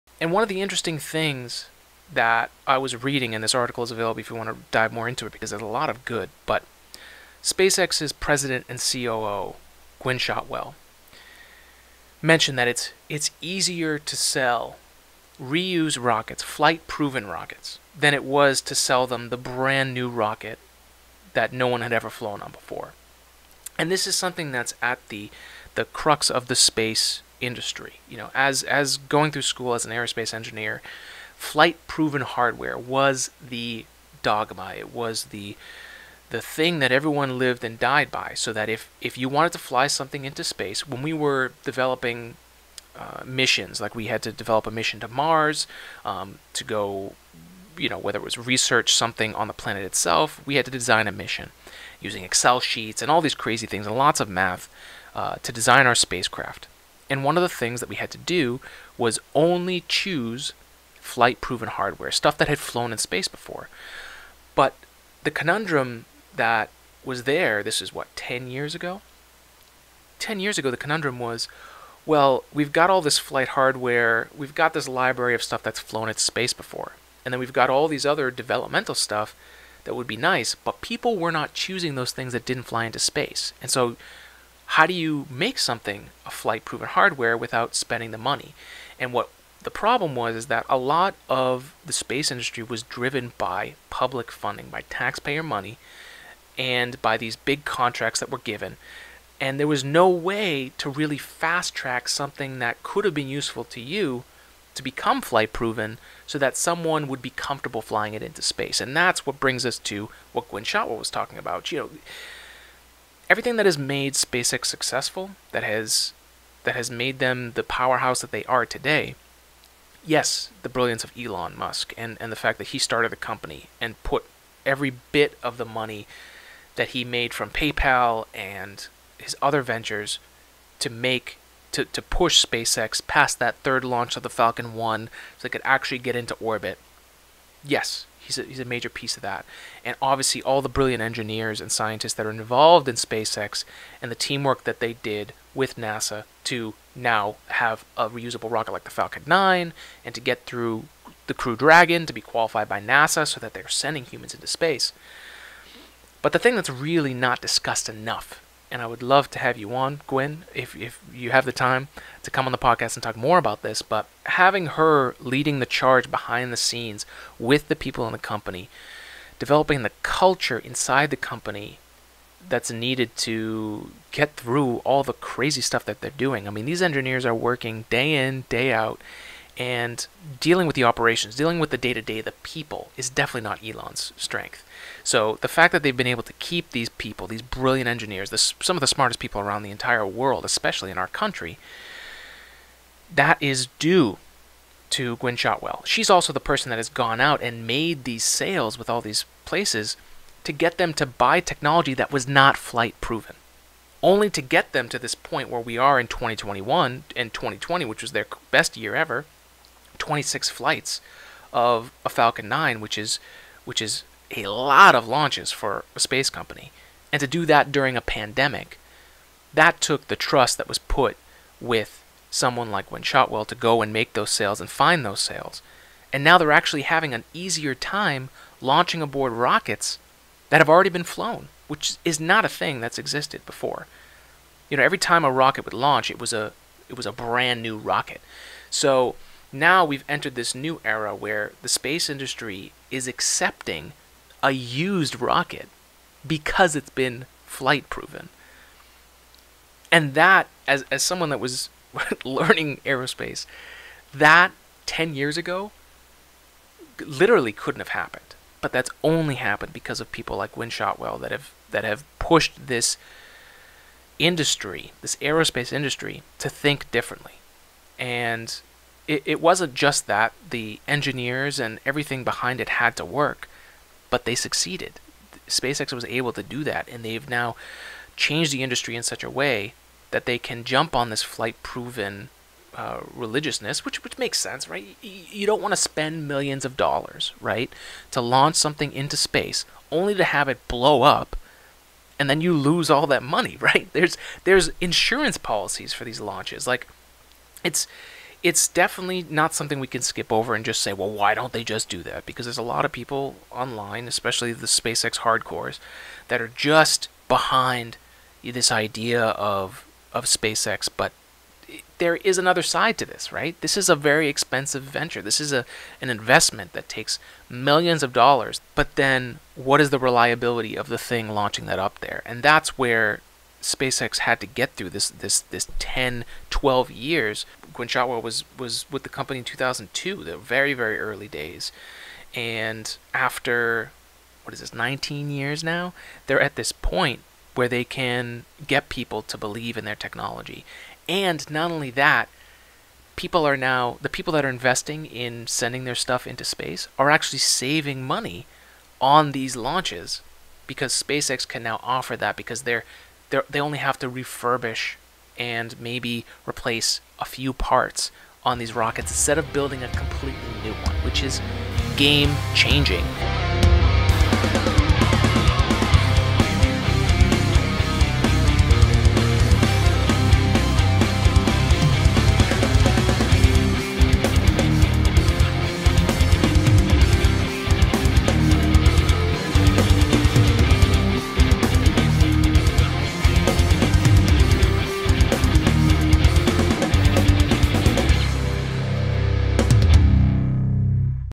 How SpaceX's Gwynne Shotwell helped change the Space Industry towards Reusability | TIS#229 clip
The unsung hero of SpaceX is Gwynne Shotwell, their President and COO. We love her! She has done so much to help change the aerospace industry and doesn't get the recognition she deserves IMO.

This is our breakdown of HOW she succeeded in creating a culture of success within SpaceX & fought through proving to a 'flight-proven ONLY' that new rockets are worth buying!